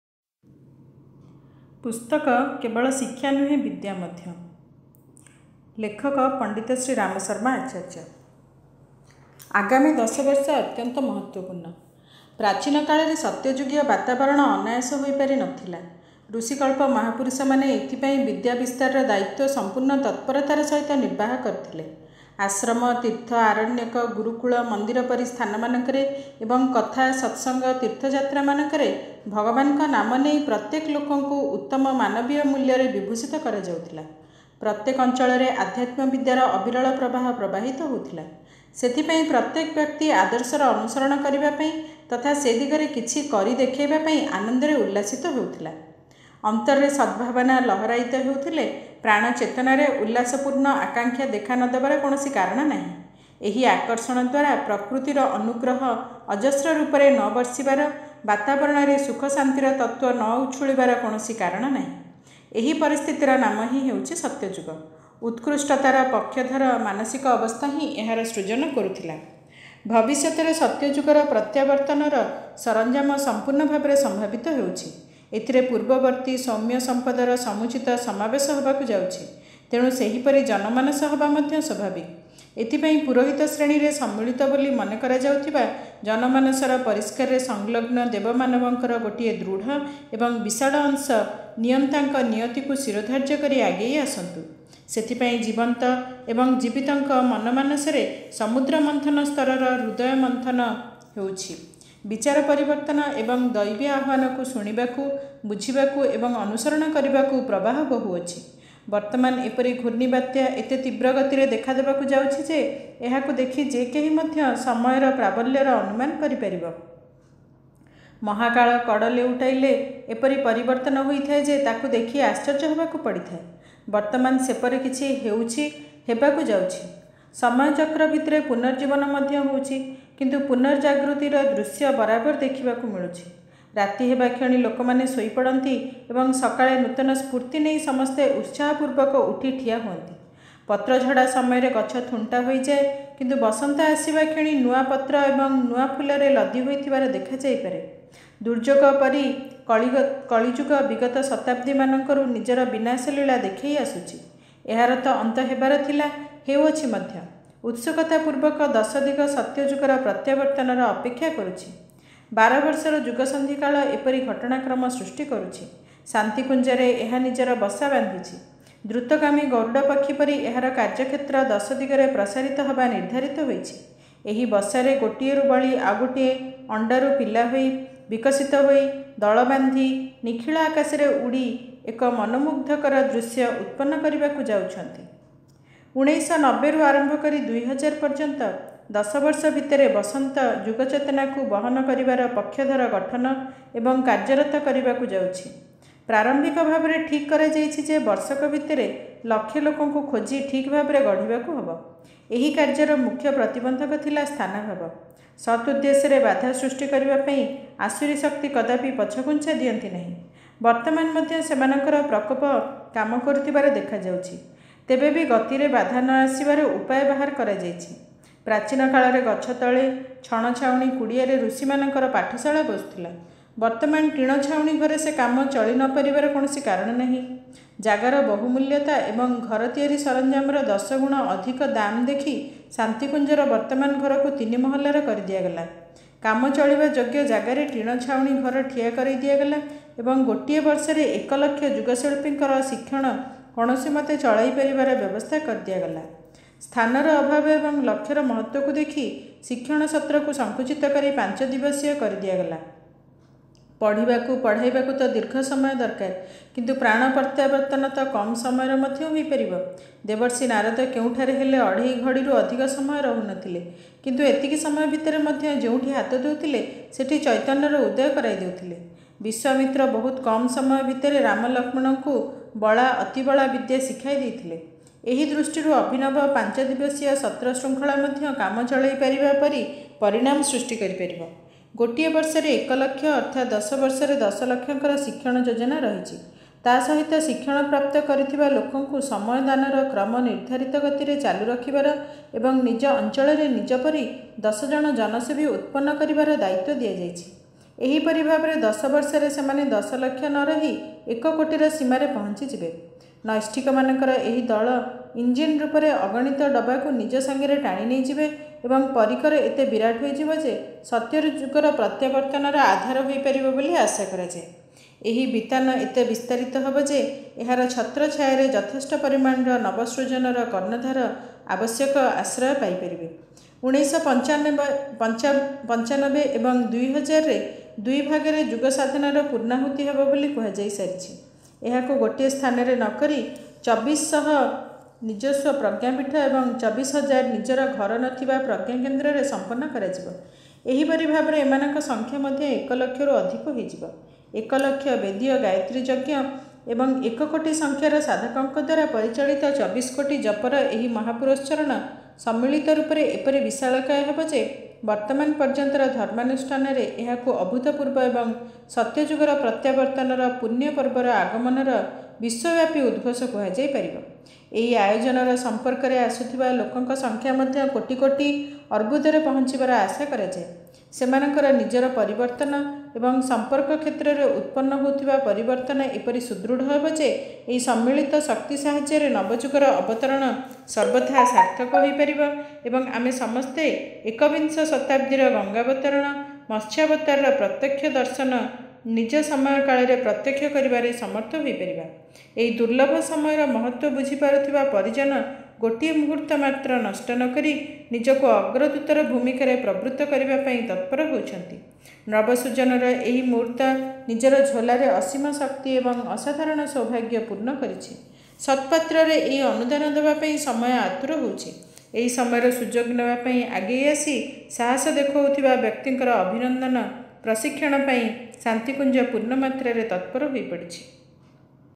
AudioBook-ପୁସ୍ତକର ନାମ--କେବଳ ଶିକ୍ଷା ନୁହେଁ, ବିଦ୍ୟା ମଧ୍ୟ Part--8 (ओड़िया)
Audio Book --ସମସ୍ୟା ଅନେକ ସମାଧାନ ଏକ  (ओड़िया)

ଲେଖକଙ୍କ ବିଷୟରେ
 ପଣ୍ଡିତ ଶ୍ରୀରାମ ଶର୍ମା ଆଚାର୍ଯ୍ୟ (୧୯୧୧-୧୯୯୦) ଯିଏ ଜଣେ ଭାରତୀୟ ସ୍ୱାଧୀନତା ସଂଗ୍ରାମୀ, ସାମାଜସଂସ୍କାରକ, ମହାନ ଗାୟତ୍ରୀ ସାଧକ, ଯୁଗନିର୍ମାଣ ଯୋଜନାର ସୂତ୍ରଧର, ଯୁଗର ଋଷି ଏବଂ ବୈଜ୍ଞାନିକ ଆଧ୍ୟାତ୍ମିକତାର ଅଗ୍ରଦୂତ ତଥା ବିଶିଷ୍ଟ ସାହିତ୍ୟର ସୃଜେତା  ଥିଲେ।  ସେ ୩୨୦୦ରୁ ଅଧିକ ପୁସ୍ତକ ରଚନା କରିଛନ୍ତି|